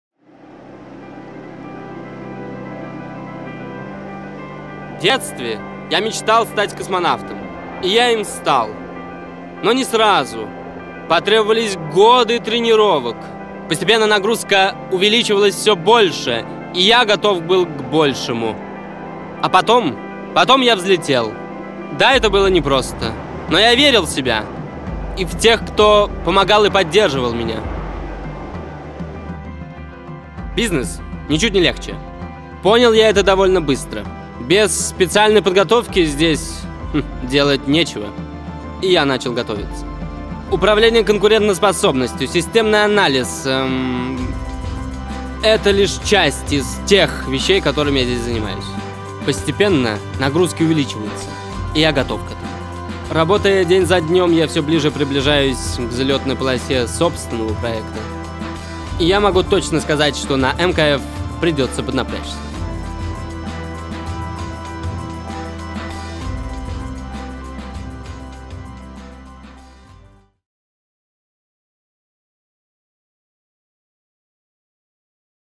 ролик Магистерского Корпоративного Факультета ИТМО
Просто рекламный ролик, забавный и со своей историей.

Во время съемок наш актер немного искупался, провалившись под лед. Мы сначала было кинулись спасать его, но осознав что там воды по пояс, и глядя на его попытки вылезти, мы не смогли ничего сделать - у нас началась истерика. 3 взрослых мужика ржали как лошади, катаясь в снегу. В итоге, конечно, мы его вытащили и долго грели в машине.